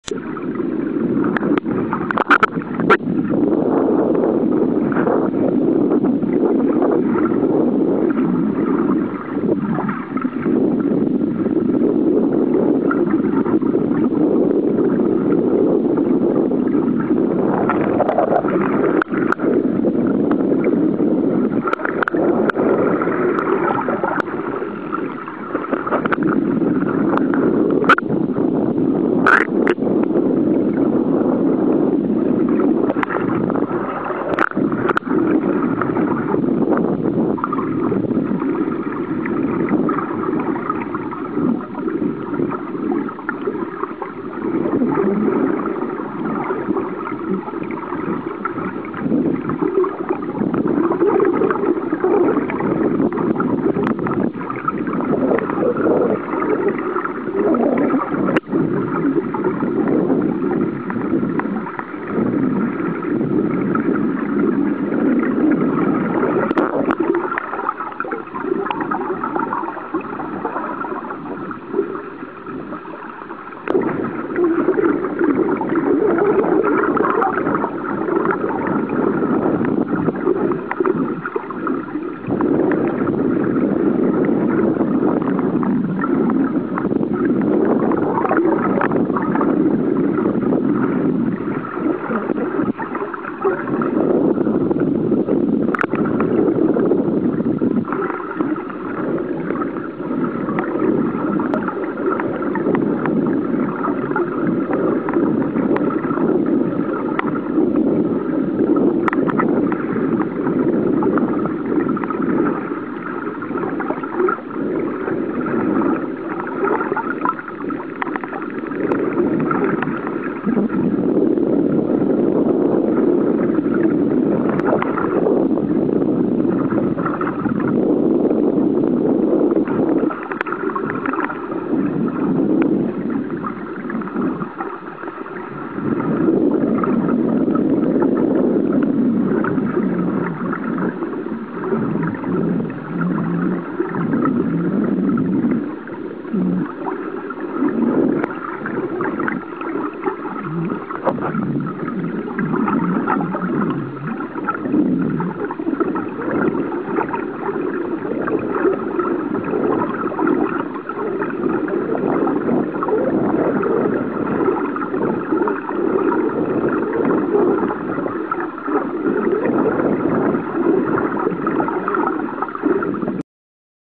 Cláudia Brum with Spotted dolphins by Ocean Defender
We were already a few miles from land in search of dolphins suddenly there they are, at least about 30 came from all sides. We decided to dive in and swim with them ... the best way to see them is in natural habitat.